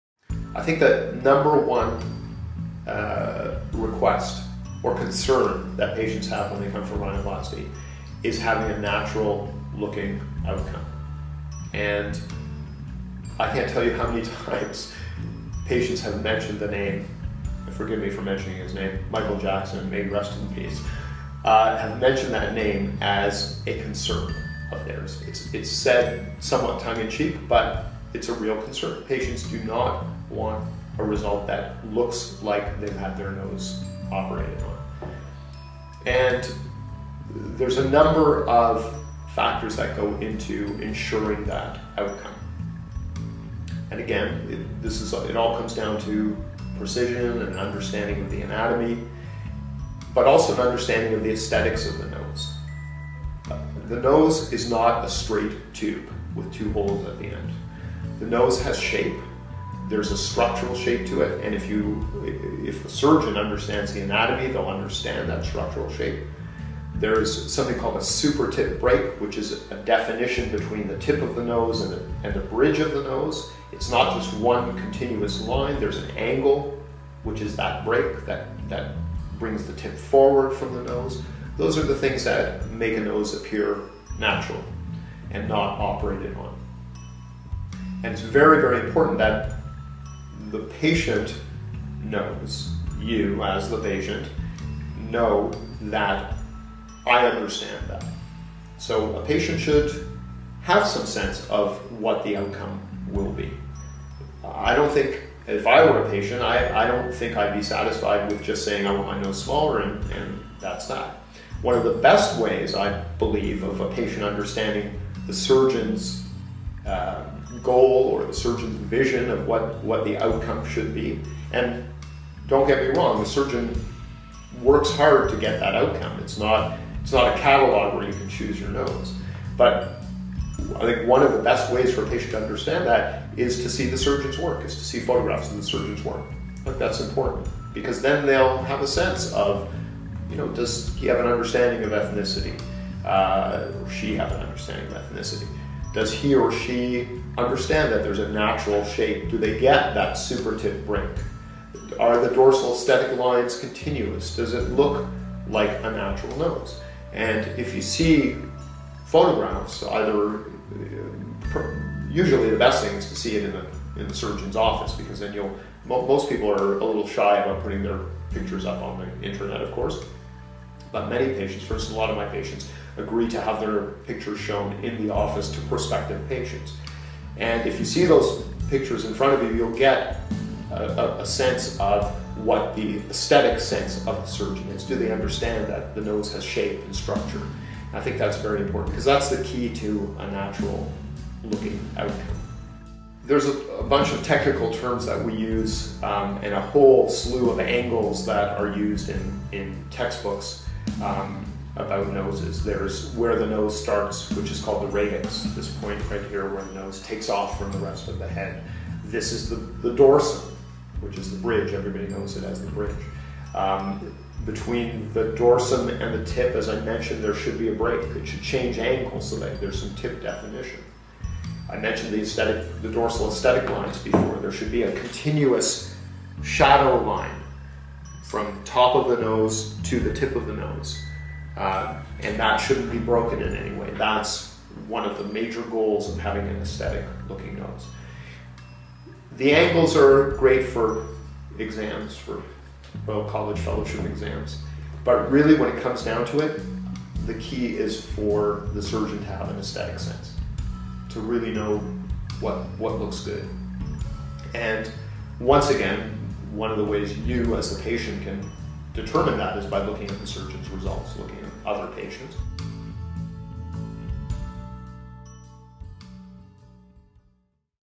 How To Get Natural Looking Rhinoplasty (Nose Job)
Get an understanding of the different structures of the nose and how they contribute to giving you a natural looking result. When getting rhinoplasty (nose job) it's important to respect your current appearance and maintain your ethnic identity.

Edelstein Cosmetic
362 Fairlawn Avenue,
Toronto, ON, M5M 1T6